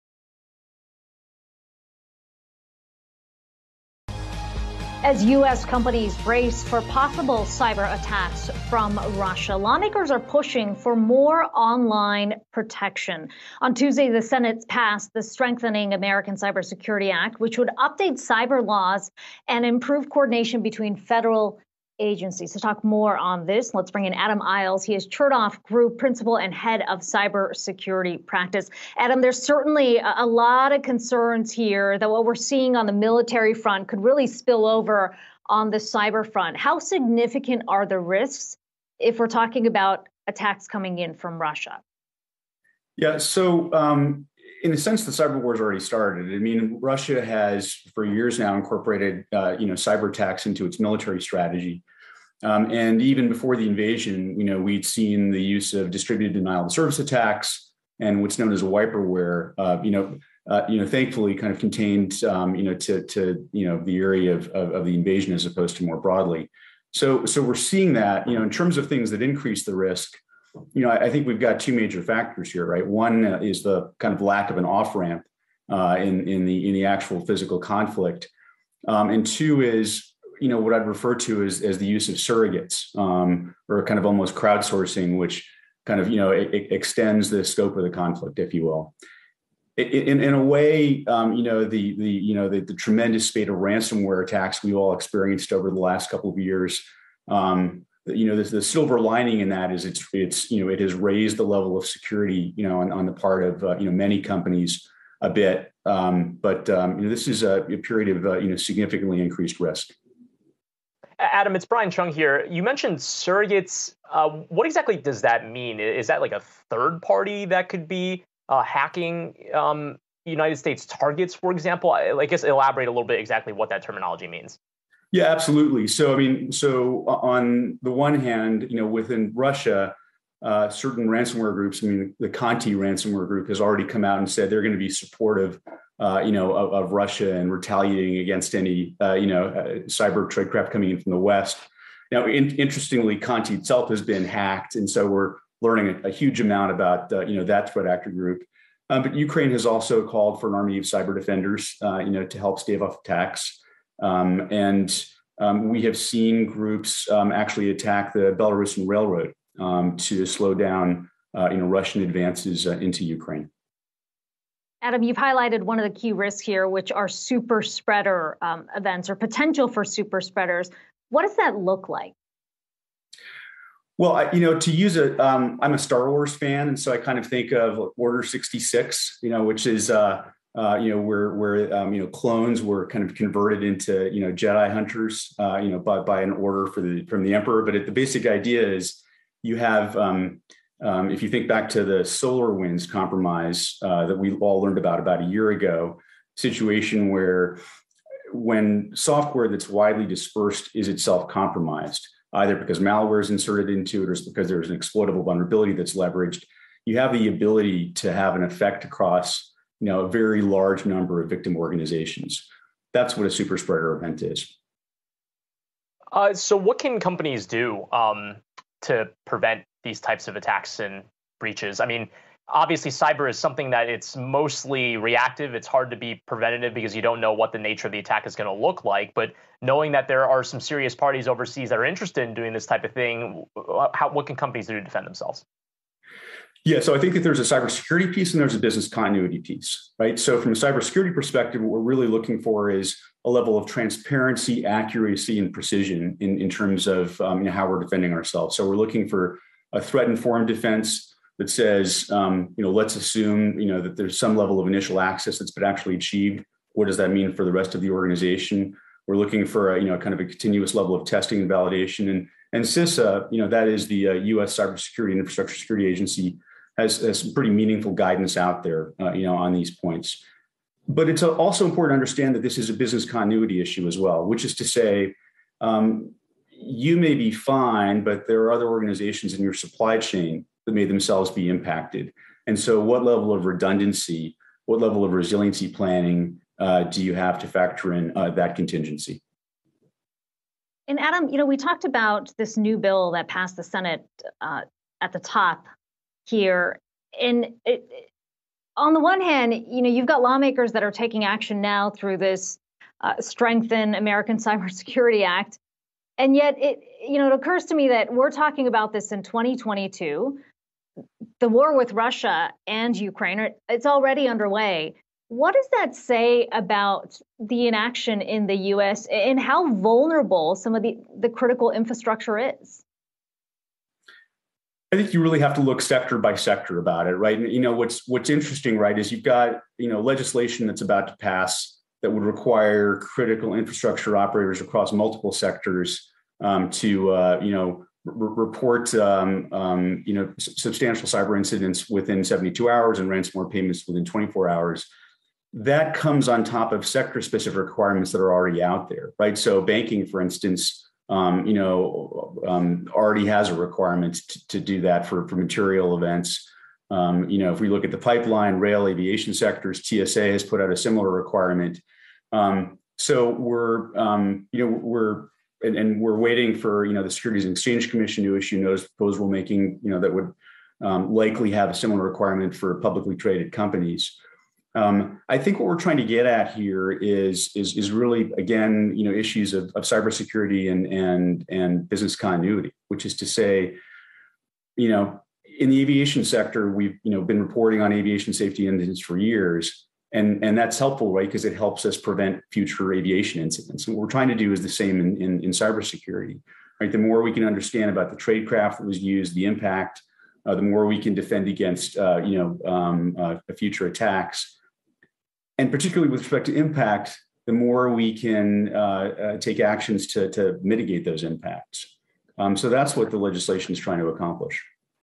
Russia-Ukraine war: 2 factors that may increase the risk of cyberattacks, according to an expert
Adam Isles, Chertoff Group Principal and Head of Cybersecurity Practice, joins Yahoo Finance Live to discuss Russia's cyberwar capabilities and the outlook for cybersecurity and ransomware attacks.